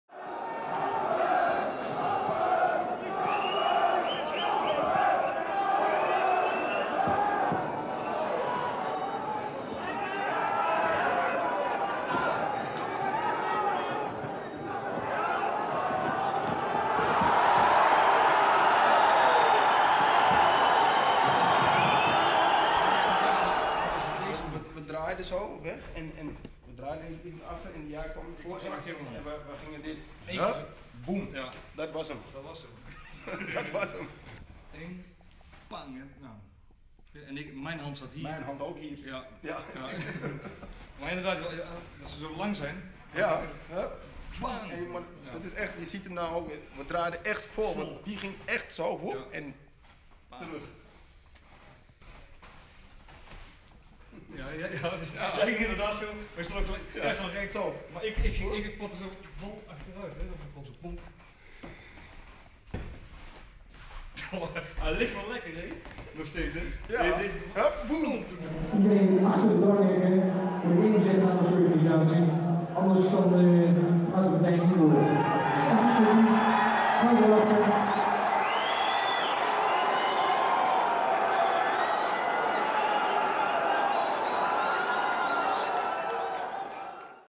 Ramon Dekkers vs Rayen Simson [Double KO]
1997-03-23 Ramon Dekkers and Rayen Simson fighting in Muay Thai Gala: The Night of War event, after a long time they met and discussed about the fight and double KO.